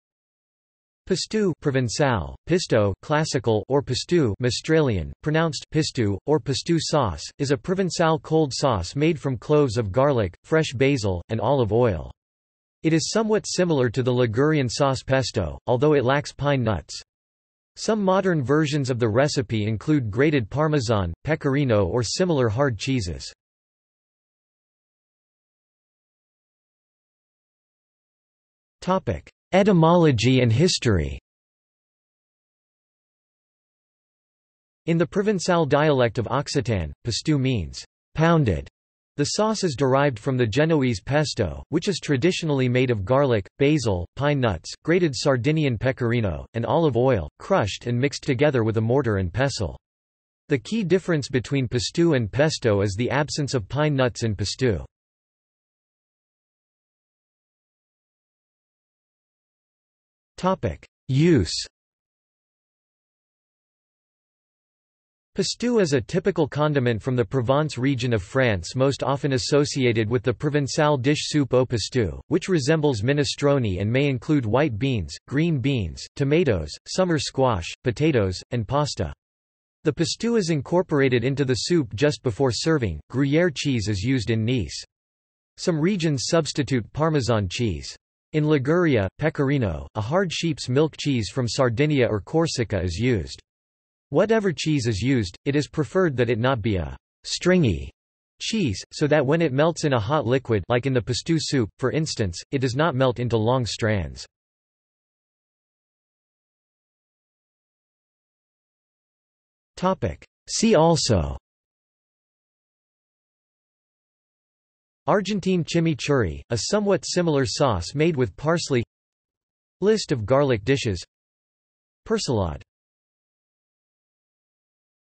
Pistou | Wikipedia audio article
This is an audio version of the Wikipedia Article:
Pistou

00:00:25 1 Etymology and history
00:00:56 2 Use
00:01:46 3 See also

- Socrates

SUMMARY
Pistou (Provençal: pisto (classical) or pistou (Mistralian), pronounced [ˈpistu]), or pistou sauce, is a Provençal cold sauce made from cloves of garlic, fresh basil, and olive oil. It is somewhat similar to the Ligurian sauce pesto, although it lacks pine nuts. Some modern versions of the recipe include grated parmesan, pecorino or similar hard cheeses.